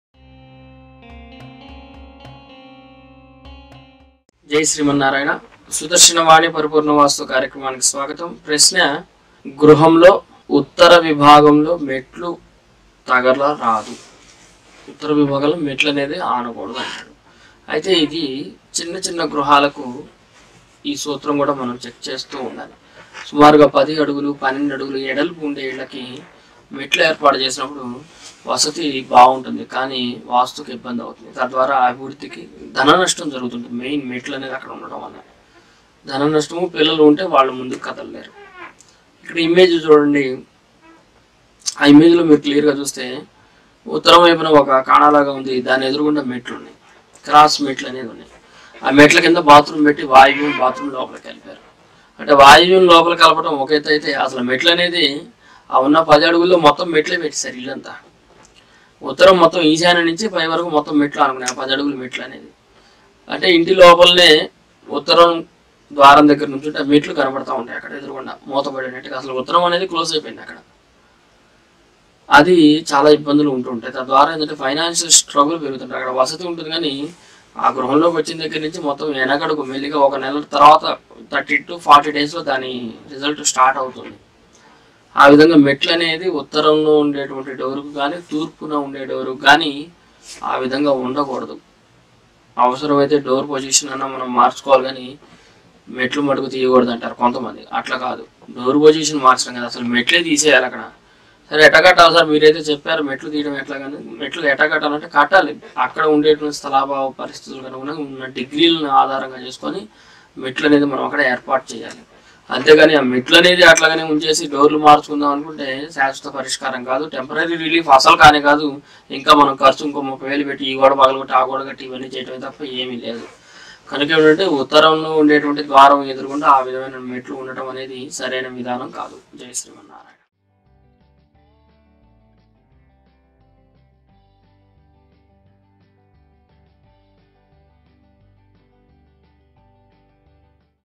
SV-0754 గృహానికి ఉత్తర భాగంలో మెట్లు ఉండరాదు - ధన నష్టం | Staircase Vastu in Telugu
గృహానికి ఉత్తర భాగంలో మెట్లు ఉండరాదు - ధన నష్టం | Staircase Vastu in Telugu | Daily Vastu Tips

Vastu Ratna Sreemaan GUDIMELLA LAKSHMI NIRANJAN SWAMI is a Hereditary vastu expert ,
He gave his interviews in BHAKTI TV and PUJA TV.